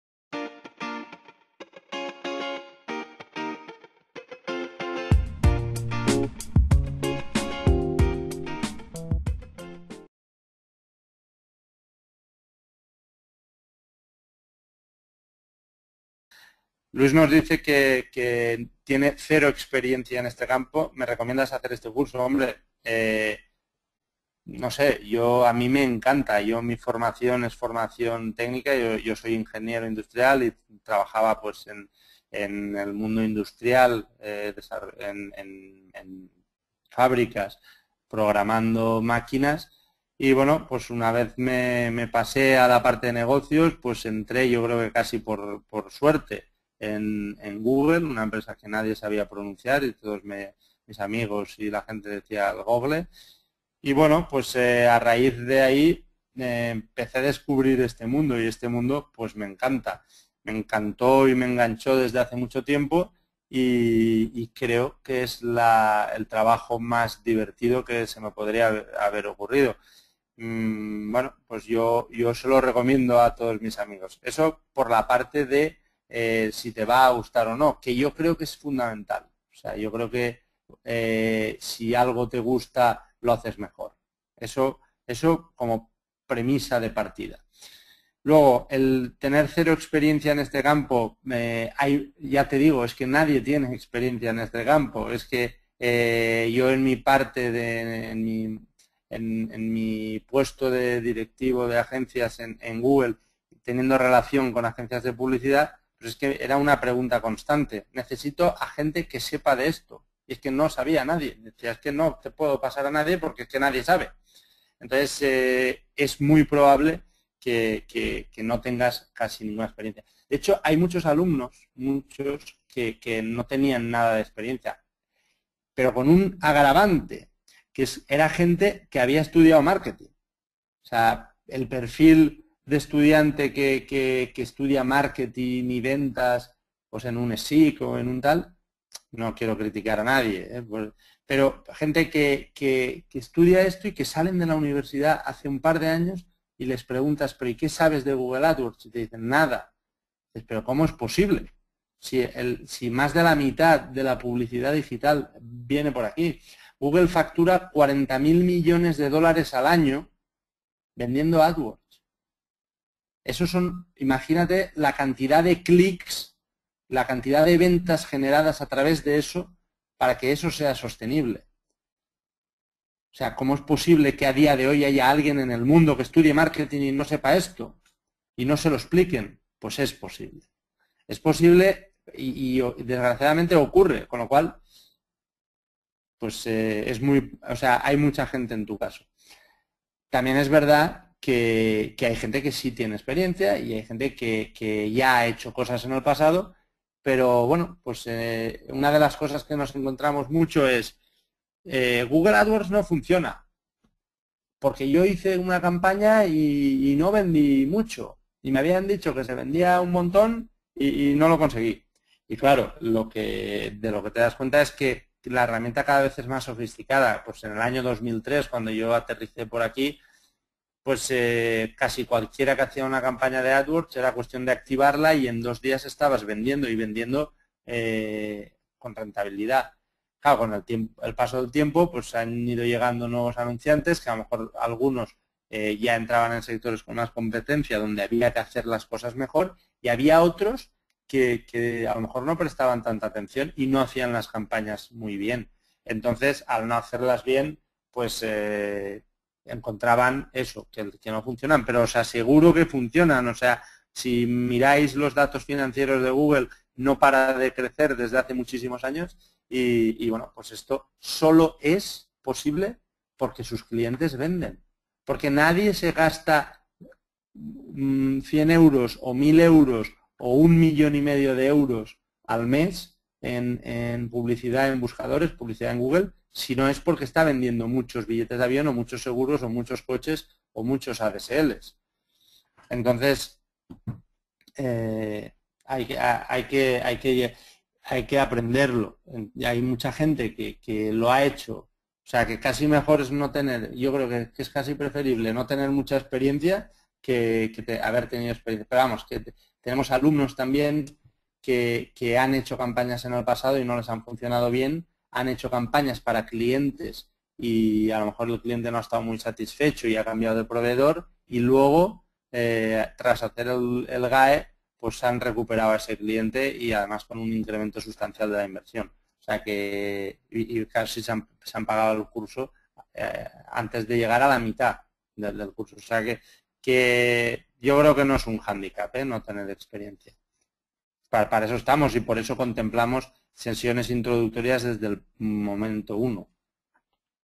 ¿Qué perfil tienen los alumnos del GAE ?
Pregunta realizada en la presentación del Programa Google Adwords Experience, por Manuel Arrufat.